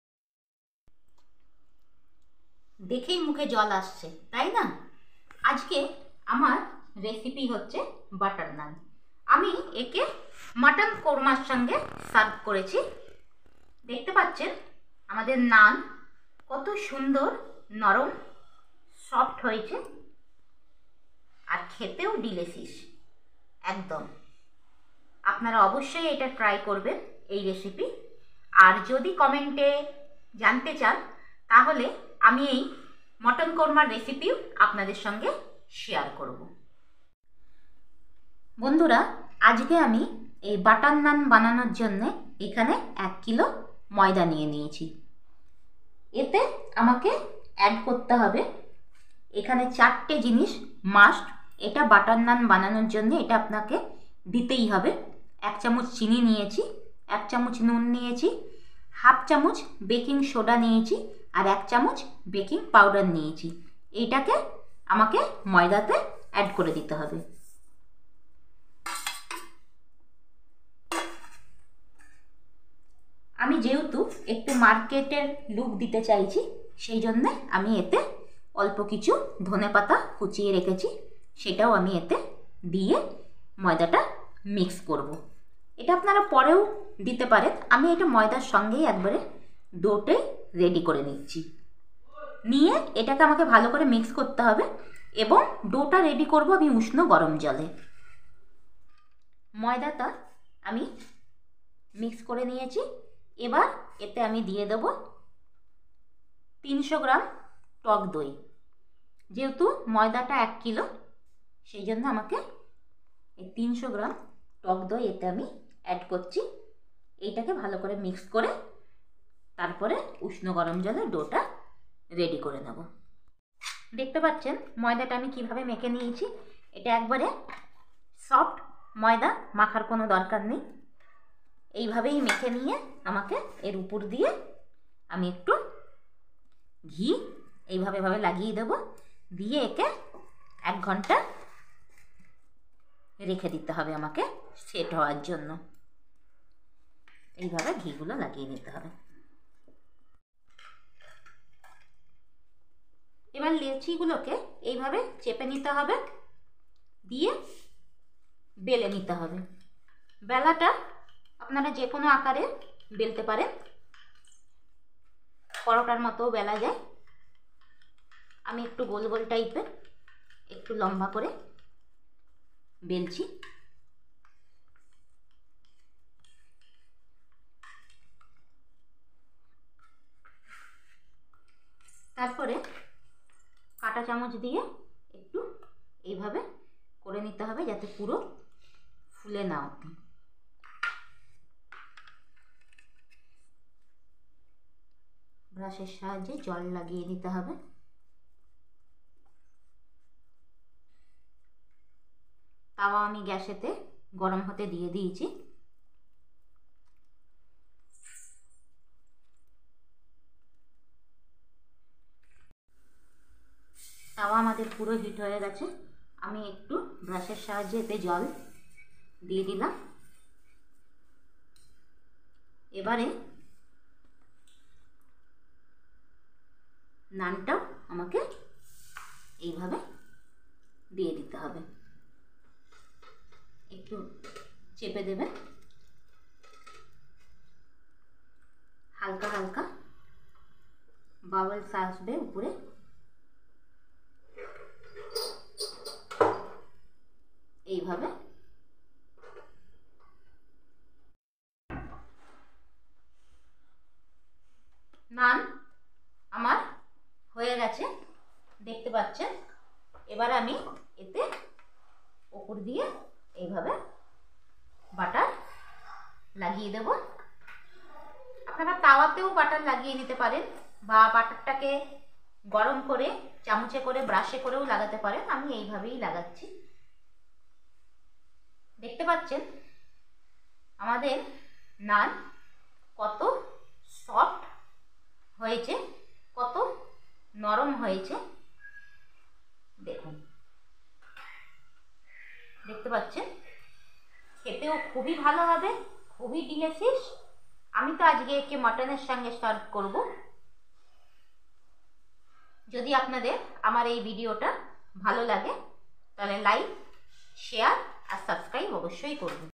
Butter Naan Recipe on Tawa !! Butter Naan Recipe !! তন্দুরি নান তাওয়াতে বানিয়ে ফেলুন !!
5:45 minute

             welcome to BT SOLUTION
             बीटी समाधान में आपका स्वागत है
               BT SOLUTION-এ স্বাগতম